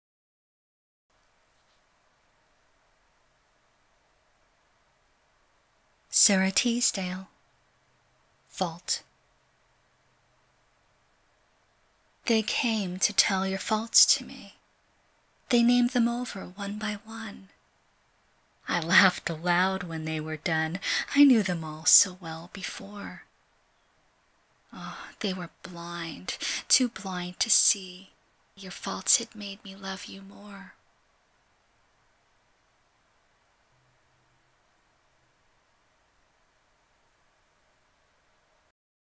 Sara Teasdale - Fault (audio with text)
Sara Teasdale (August 8, 1884- January 29, 1933) was an American poet who is commonly recognized for her collection of work Love Poems, which won a Pulitzer Prize. She wrote lyrical poetry, which expresses personal emotions and observations.